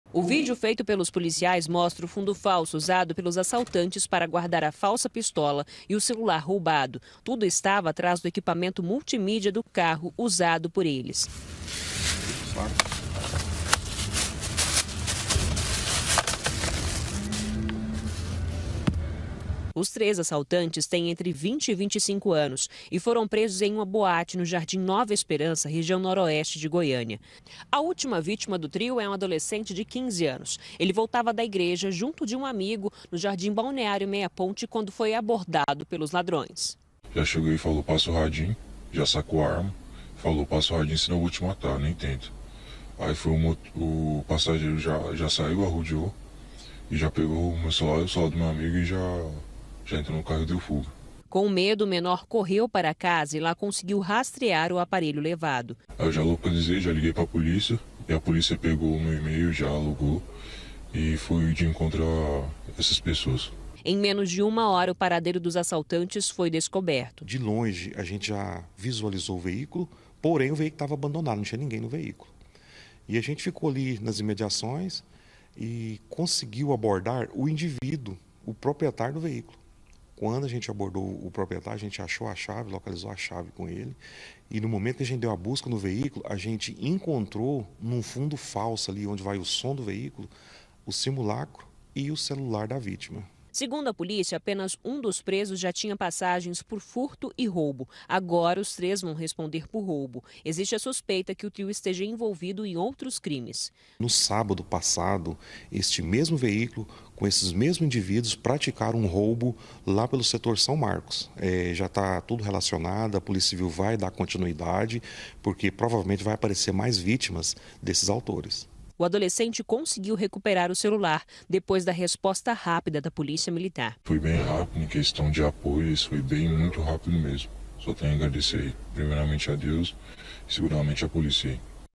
NA FORÇA DO GOGÓ: ARMA FALSA E TERROR DURANTE ROUBOS POR TRIO
TRIO COM ARMA FALSA CHEGAVA NO GRITO, TOCAVA O TERROR COM AS VÍTIMAS E LEVAVA O QUE CONSEGUIA. ELES SÓ NÃO ESPERAVAM QUE O ÚLTIMO A TER UM CELULAR ROUBADO ERA MAIS INTELIGENTE. O ADOLESCENTE ACHOU O LOCALIZADOR DO APARELHO, E DEU A DICA DE ONDE OS CRIMINOSOS ESTAVAM PRA A POLÍCIA. NO FINAL, VOCÊ DEVE IMAGINAR QUEM SAIU NO PREJUÍZO.